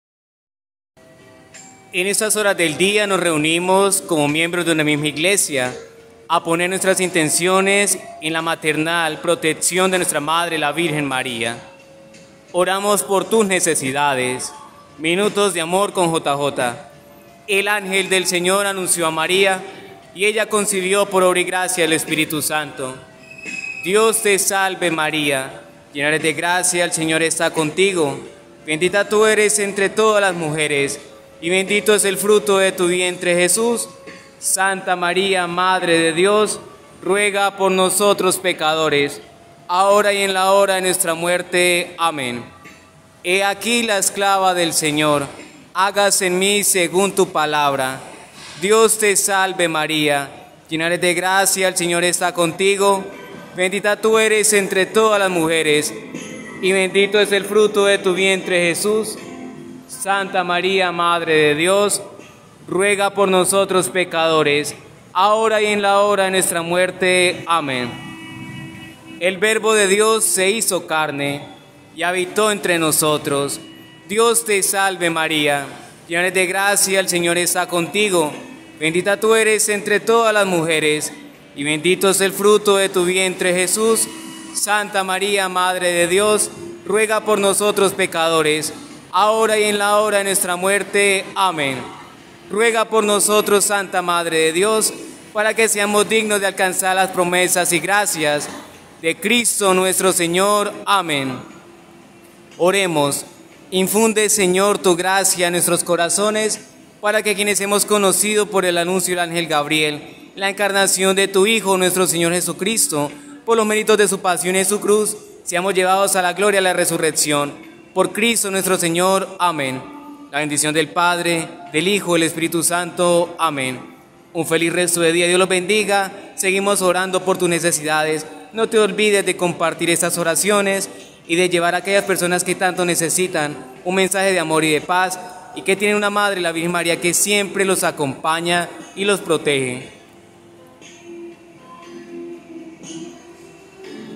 Ángelus para este día, Pedimos por los más necesitados enfermos y los que sufren
En este día virgen María pedirte por aquellos que sufren en cuerpo y alma. 🙏😇💪